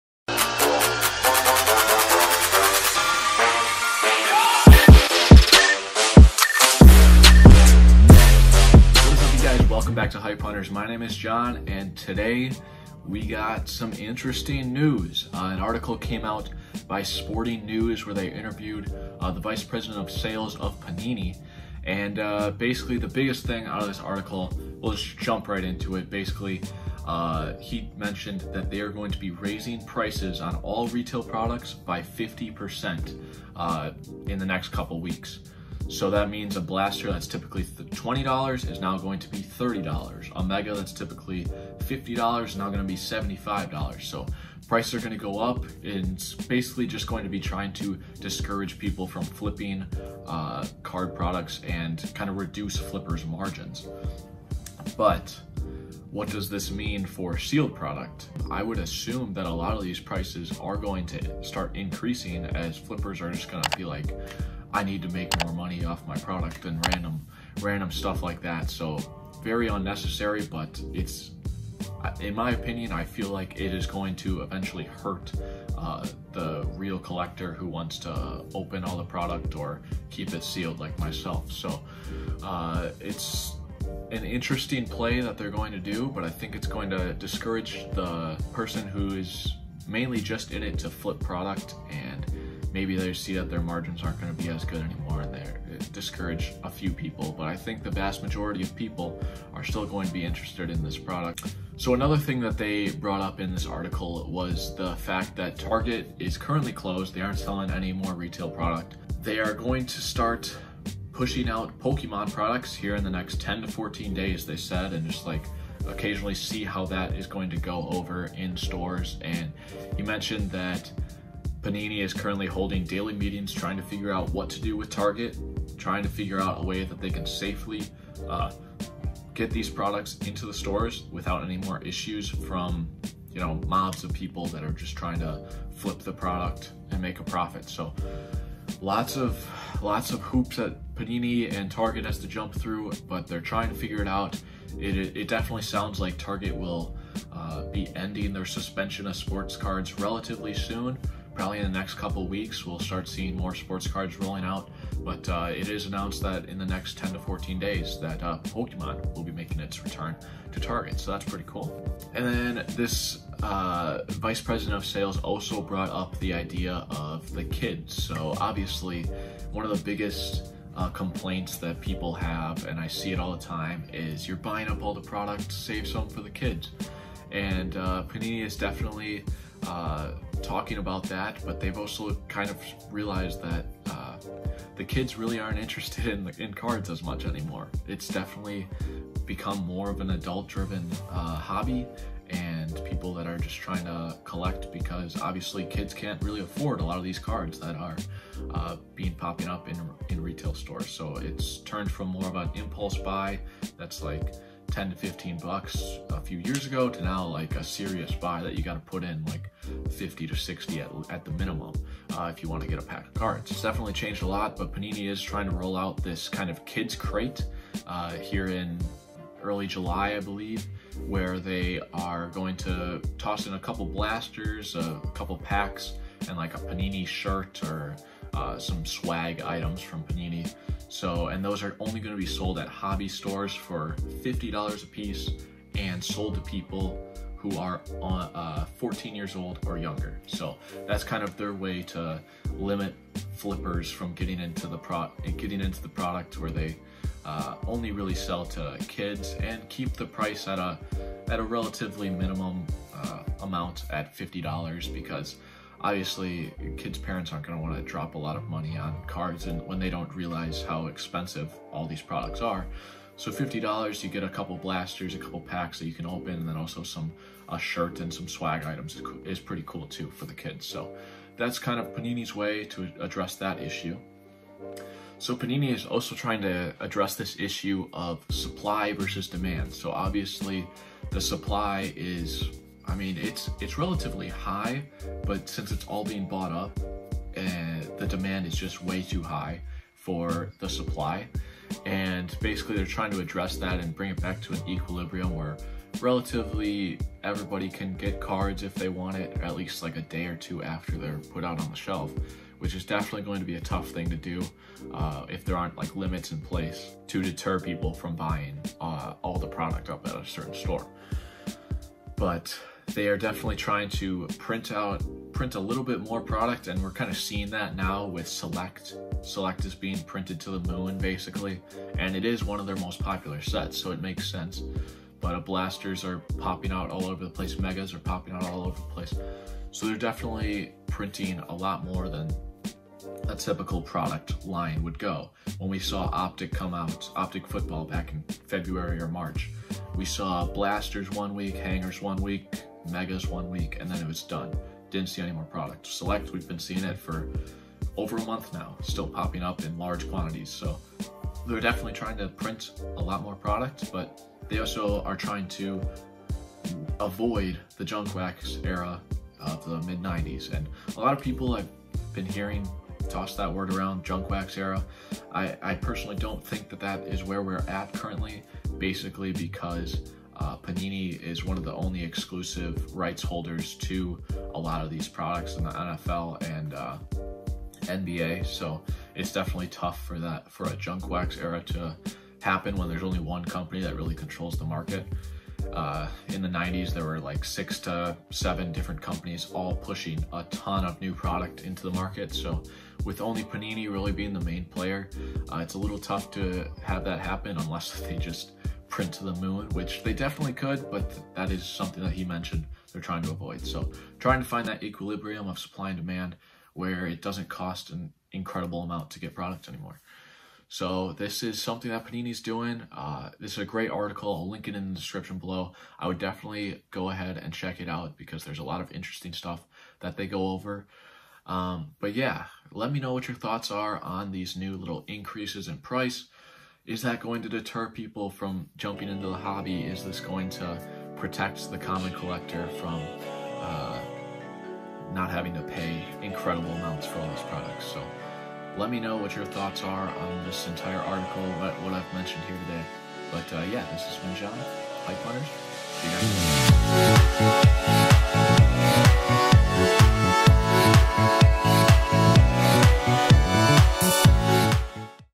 RETAIL CARD PRICES INCREASING 50%! (Target to stock Pokemon Products soon, Kid's Crate, and more!)
In this video, John discusses the increase in retail card products that was announced by the Panini VP of Sales earlier today! This means that all cards will be increasing in price by 50%. Will this hurt or help the hobby, I don't entirely know. Let me know your thoughts on this change or anything I mentioned in today's video in the comments below!